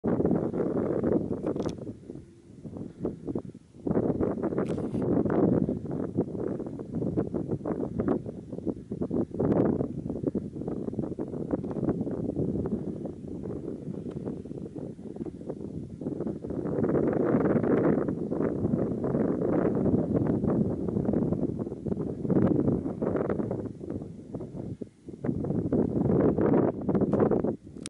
Raptor. Bird of Prey Hovering
Raptor initially hovers above Strathblane Hills for a bit then swoops off west at great speed. Then turns back and heads NE. Not sure what it was so if anyone knows? The stunning flight speed may give a clue! It`s a bit distant which made if difficult to ID.